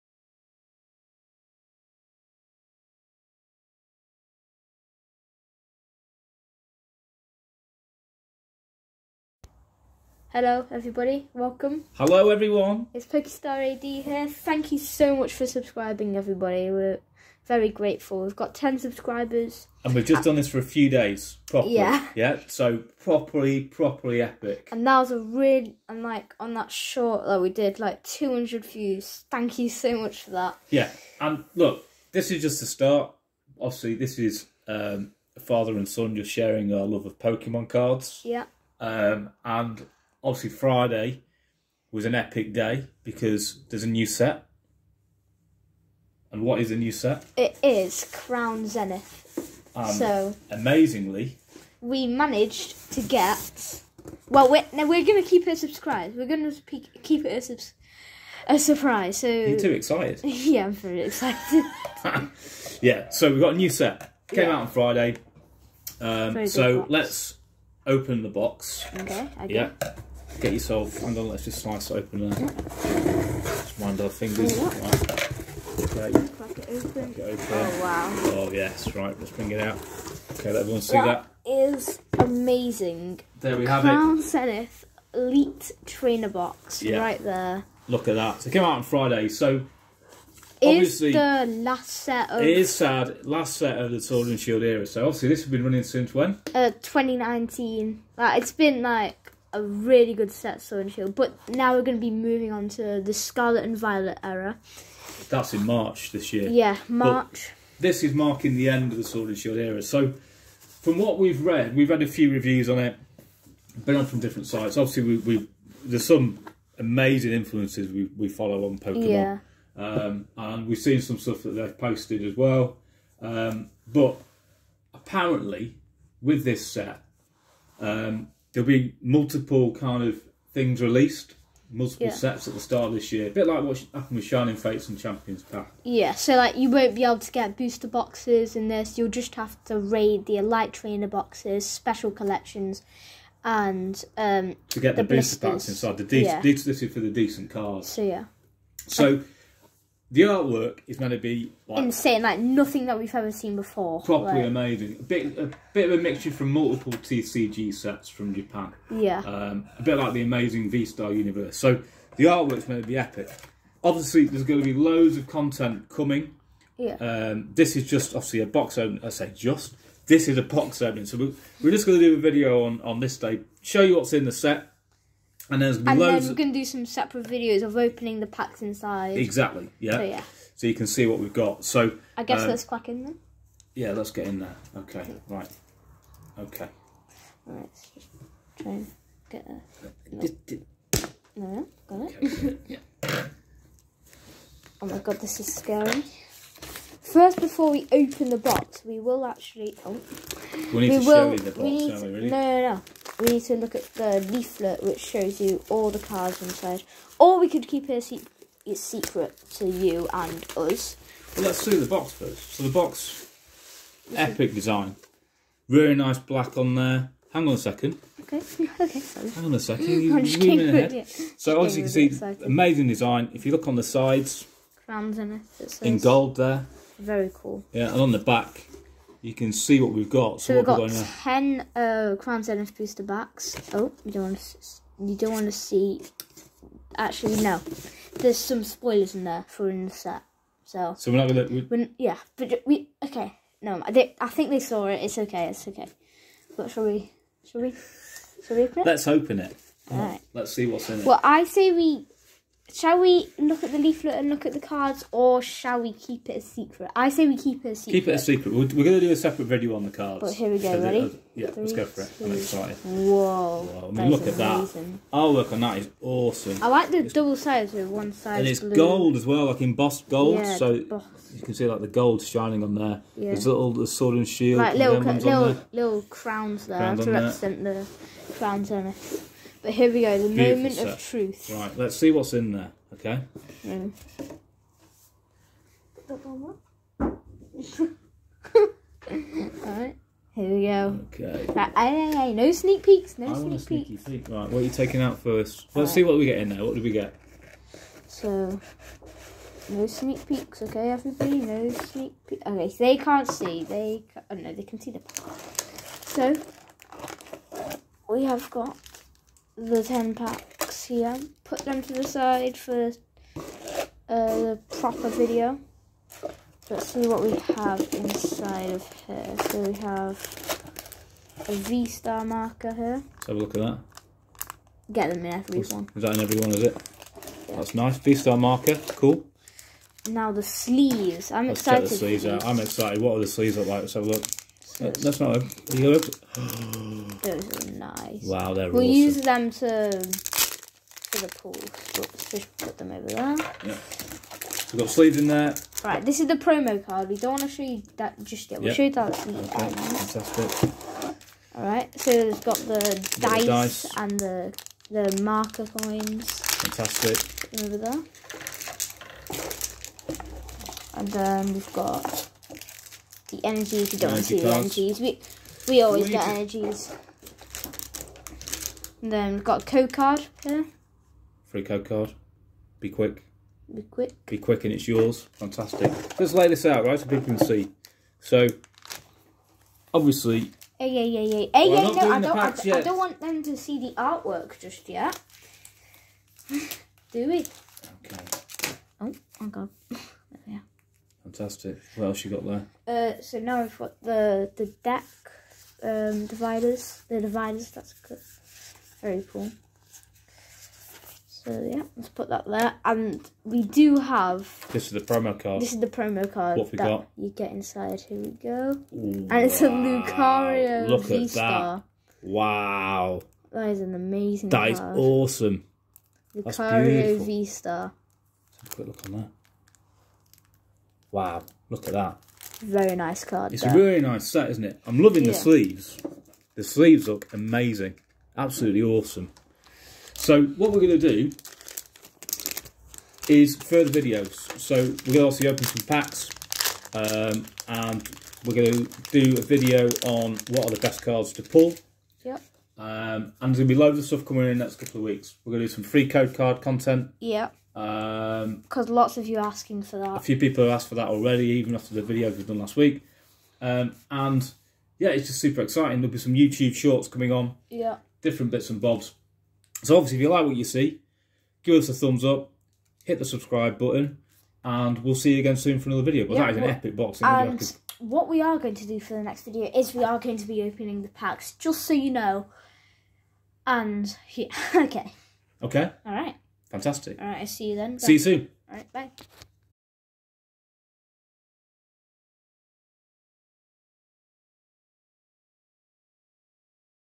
Pokemon Crown Zenith Elite Trainer Box Opening...YOU believe what's inside!
We're so excited to open and share what's in the Crown Zenith Elite Trainer Box, Pokemon's latest set!

Crown Zenith pack Opening videos coming soon!